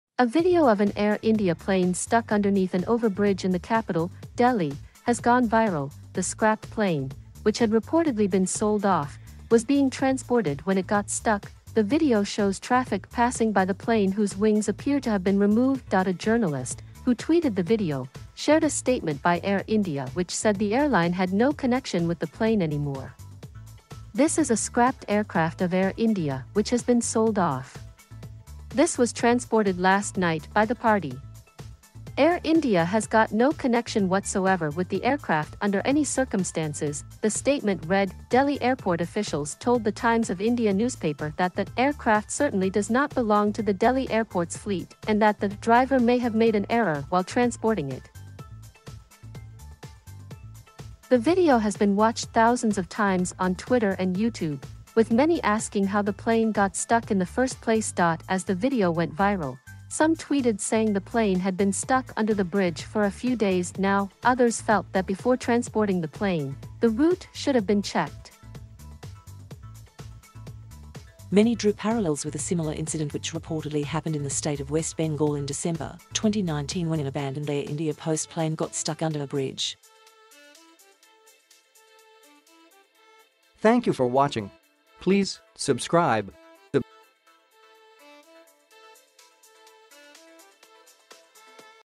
Air India: Plane stuck under bridge in viral video
News of the world: Air India: Plane stuck under bridge in viral video. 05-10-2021

A video of an Air India plane stuck underneath an over-bridge in the capital, Delhi, has gone viral.The scrapped plane, which had reportedly been sold off, was being transported when it got stuck.The video shows traffic passing by the plane whose wings appear to have been removed.A journalist, who tweeted the video, shared a statement by Air India which said the airline had no connection with the plane anymore."This is a scrapped aircraft of Air India which has been sold off. This was transporte

me-and-my-illusion.